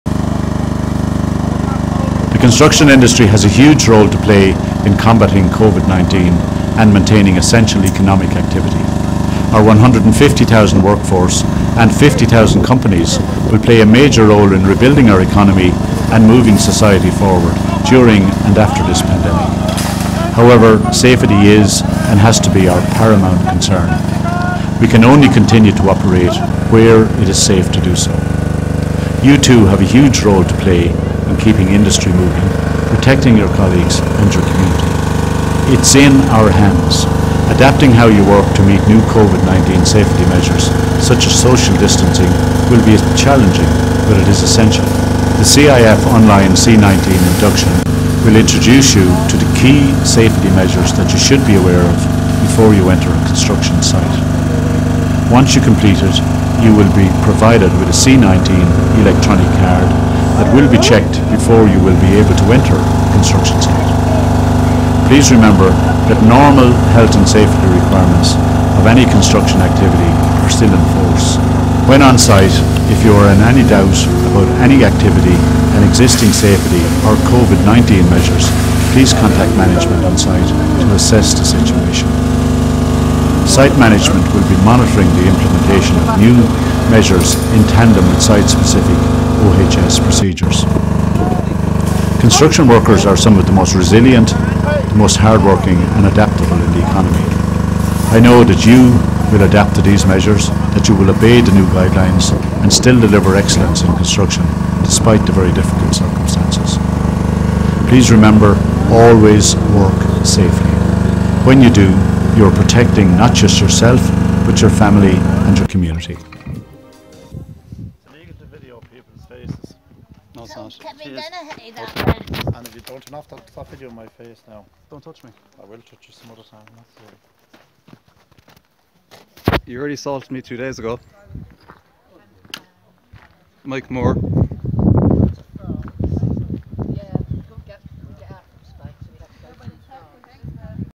Covid-19 Site Health & Safety Demonstration by Suir Engineering and Cerberus Security
We learned how Suir Engineering and Cerberus Security are enacting their new Covid-19 health and safety measures during phase 1 of the plan to get back to work in Ireland during the Coronavirus pandemic. They might be able to loosen up a bit after phase 2 kicks in...

OK seriously:

This is a short video of some of the events of the 2nd of June at Fruit Forest Farm, where there is a conflict between Keel Energy which is a wind farm developer and local landowners. Complaints have gone into the HSA and the Gardaí, but it will probably take a few days for them to act. We need support at the camp 24/7 for the next few days. Joe Franz owns that roadside and welcomes all to camp or park. Just bearing witness even for a short time is a huge help. And spread the word too by sharing these videos please.

I'll post a longer introduction to the conflict written two weeks earlier below.

Talk to Joe Franz, the land owner objecting: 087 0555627

Suir Engineering Ltd
Unit 9A Cleaboy Business Park,
Old Kilmeadan Road,
Waterford, X91 PX92,

 Cerberus Security Ltd, 8 Coburg St, Cork City

‘No 110 KV cables on our land’

Cables are being laid underground on Joe and Diane’s land at Carrigarierk, West Cork. The company Keel Energy has not asked their permission to lay these cables, and have ignored the fact that Joe and Diane live here when they submitted their proposal for the massive substation that they are erecting beside their land. The purpose of the substation is to be to transform the energy from the windmills which they want to build on Shehy Mór (the mountain which I grew up beside). The windmills from Shehy all across the back of Shehy to Barnadevane and beyond are too close to people’s homes, disrespecting biodiversity regulations, ignoring the opinions and objections given by many of the local people, and their construction and subsequent use creates a loud vibration which disrupts the daily lives of those who never chose to live close by, also lowering the value of their homes.

If anyone lives along the cable route, the least you should be gaining from this project is financial compensation (the cables are going under the road- if they are on the side of the road that borders your land, you can refuse them or at least demand the compensation that is lawfully yours- a fact that has not been made transparent, that is cheating you out of what is yours).

Joe and Diane welcome support. There are friends down there every day to show that they see the destructiveness and indifference exhibited by the company, toward people like Joe and Diane, who live off the grid and have had their existence denied by those with more power and money than they deserve. If anyone has any ideas of something they can do to help show support, please do. Anything creative, music, camping or food is welcomed (while maintaining social distancing of course).

If anyone has a beaten up trailer/ caravan that they would risk, Joe asked if it could be placed just before where they are standing in the photo- in order to increase their physical presence on the land.